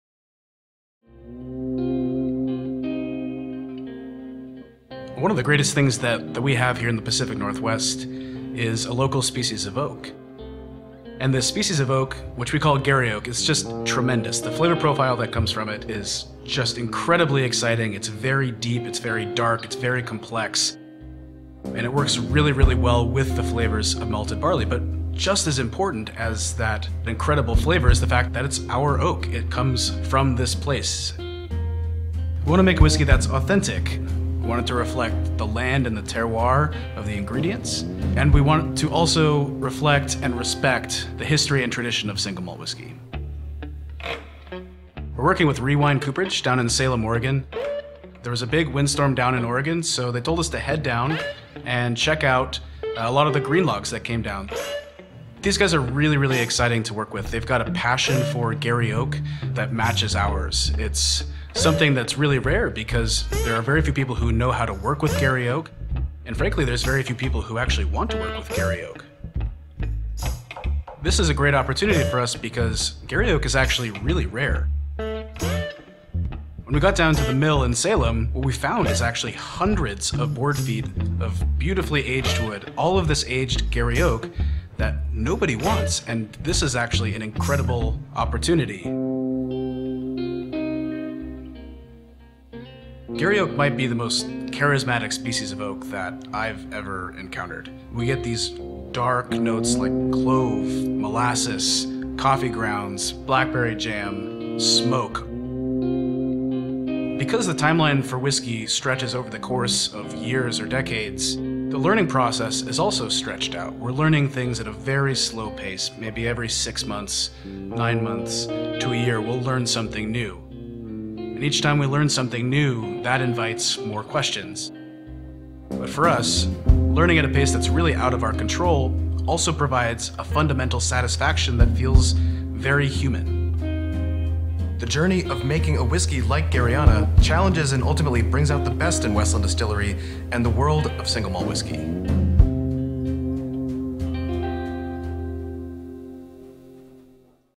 Westland Garyana
Produced for Westland Distillery and Curator Pictures. We went out to a strand of garry oaks and visited the barrel makers who make garry casks for Westland.